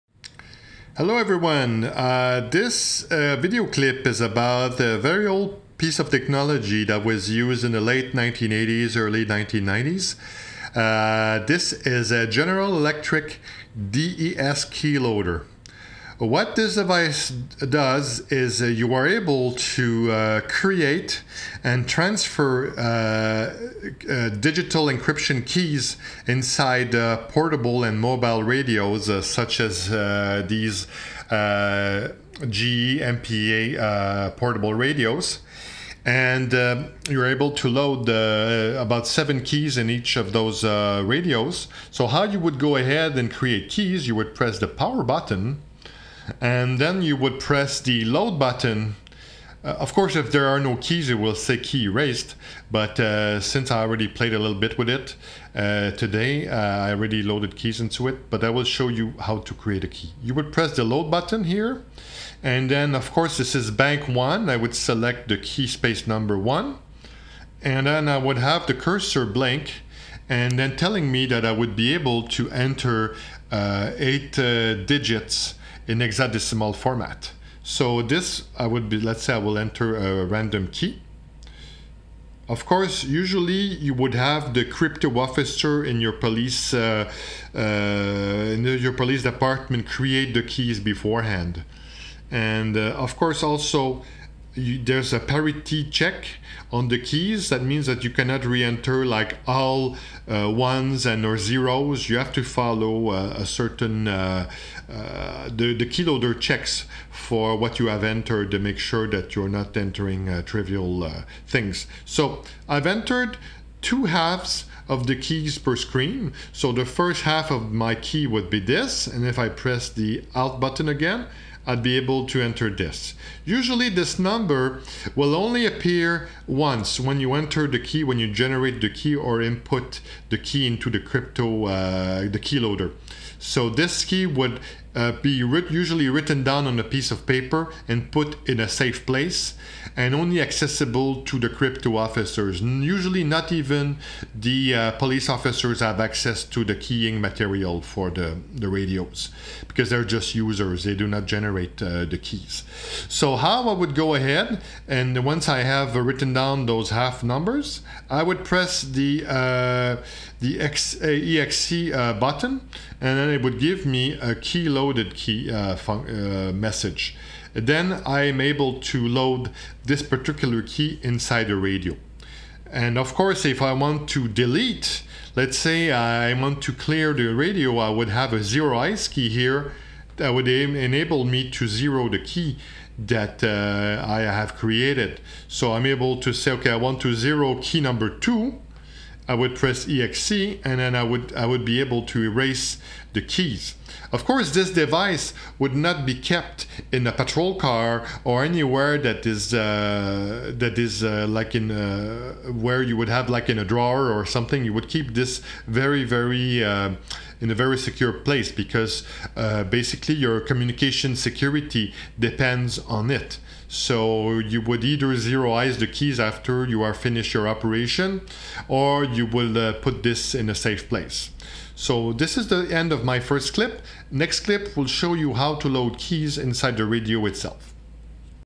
Digital encryption with old police radios - Part 1 (Keyloader)
This is part 1 of a series covering early digital voice encryption methods used by police departments across North America in the late 1980s and 1990s. This part covers encryption key generation, storage and zeroization with the General Electric DES keyloader for Voice Guard and Aegis digital modes.